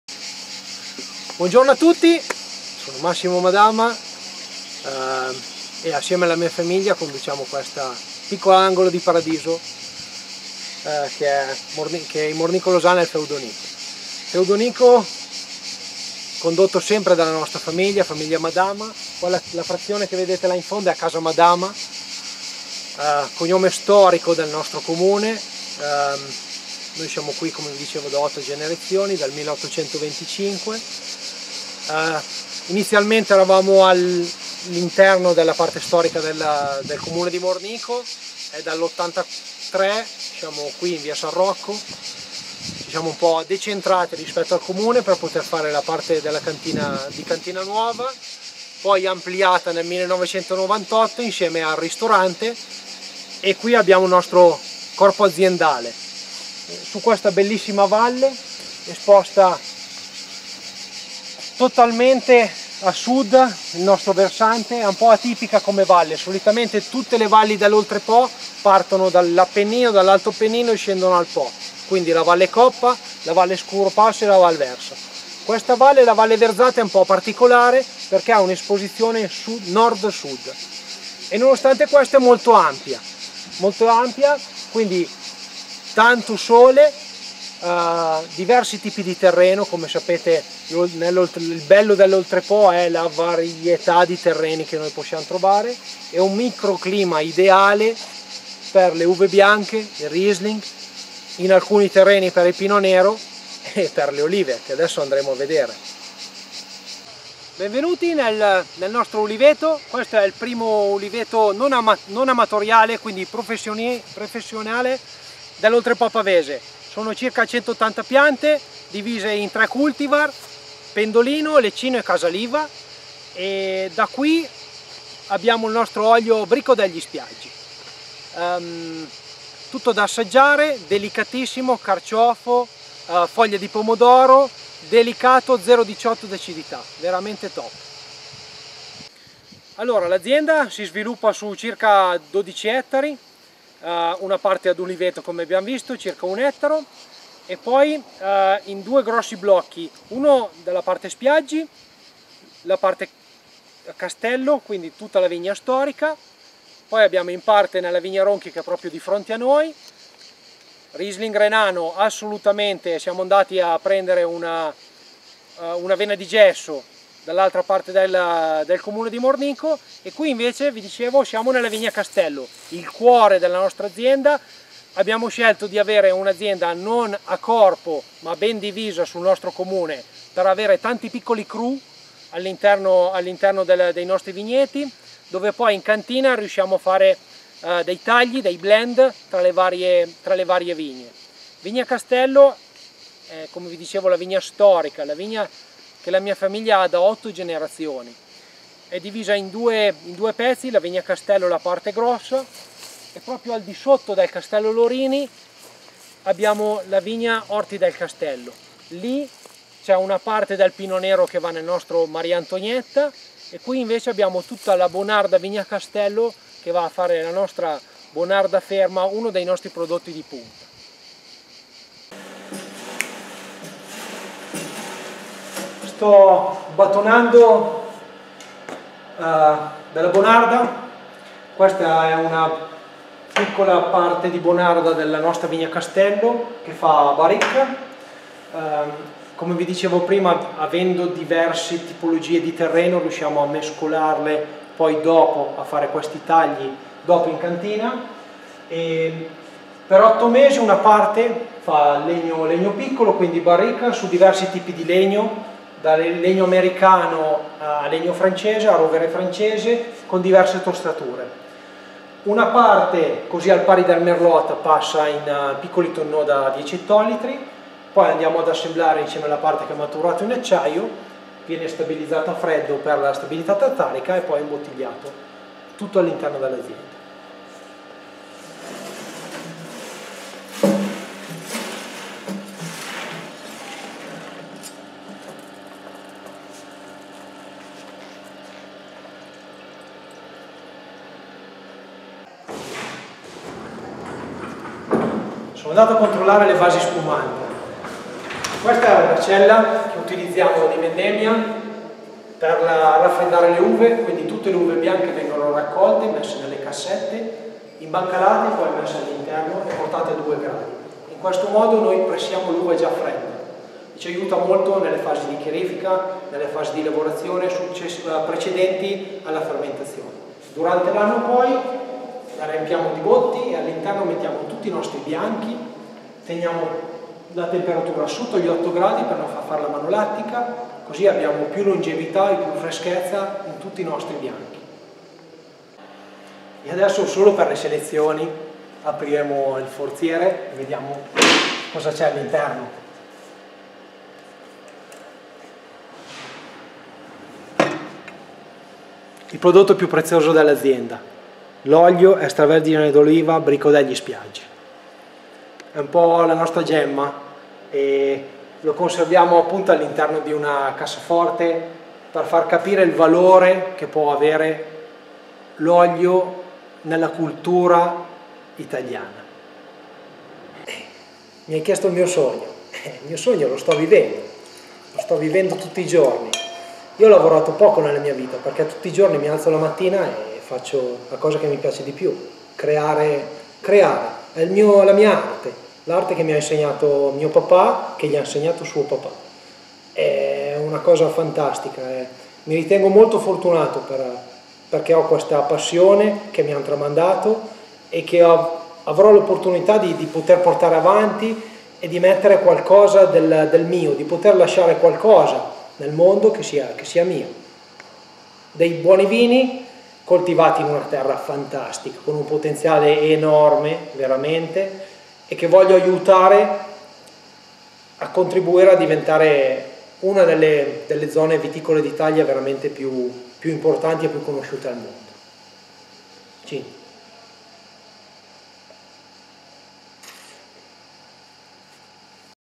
Le Selezioni dell'Oltrepò Pavese con Massimo Madama
Azienda Il Feudo Nico a Mornico Losana

Siamo in una valle veramente unica per il suo clima e morfologia, la valle Verzate, che da Torricella Verzate porta a Mornico Losana. Ed è prorprio qui, sulle alture del piccolo paese, che troviamo. vassallo del feudatario del loco.

La famiglia Madama, cognome storico in questo comune, conta ben otto generazioni di agricoltori partendo dal lontano 1825. E' infatti una delle poche aziende con oltre 100 anni di attività.

L'impulso all'azienda viene dato negli anni '70 da Edoardo Madama che, seguendo. con sistemi di vinificazione più avanzati. Oggi, accanto al padre Edoardo, nella conduzione dell'azienda c'è il giovane Massimo.

Nel segno della riqualificazione, nel 1983 la creazione di un ristorante panoramico, dove mamma Gabriella propone ricette del territorio da abbinare ai vini aziendali.
Tutte le fasi della .
La produzione si aggira sulle 50.000 bottiglie, che si divideranno in buona parte il mercato italiano e quello estero come Germania e Svizzera.

Al Feudo Nico, troviamo anche un'uliveto professionale con 180 piante di 3 cultivar diversi: leccino, casa liva, pendolino. Il micro-clima.
Tutta la loro storia sulla Guida delle Selezioni dell'Oltrepò Pavese